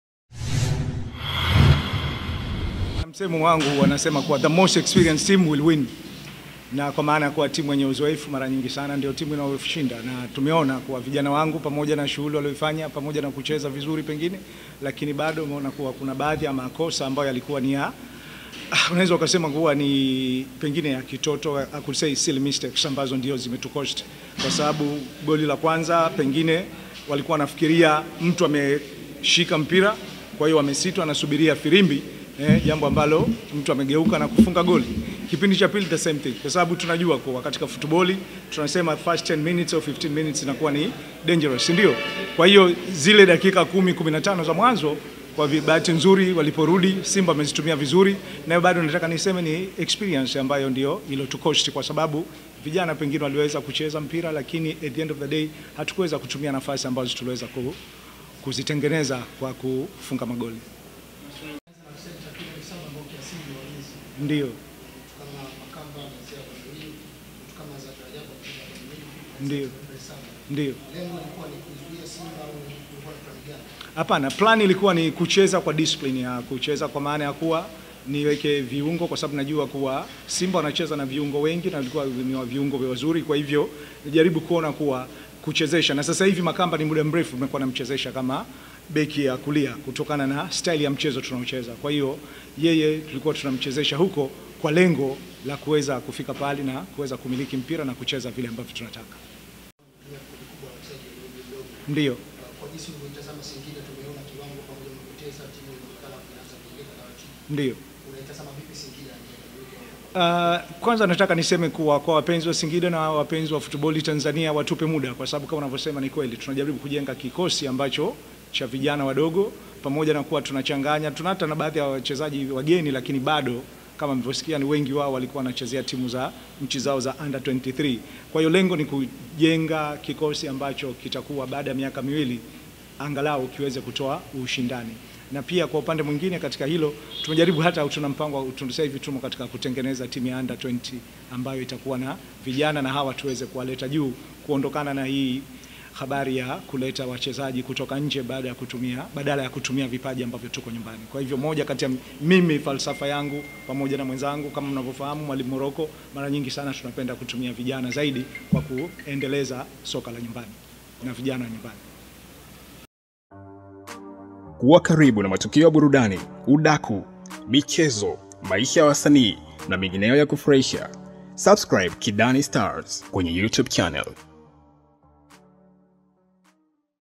Kocha Singida:Simba Wametuzidi Uwezo, Tumefanya Makosa Ya Kitoto
SIMBA SC imeonyesha bado imo kwenye mbio za ubingwa wa Ligi Kuu ya Tanzania Bara baada ya ushindi wa mabao 3-0 dhidi ya Singida United jioni ya leo Uwanja wa Taifa mjini Dar es Salaam.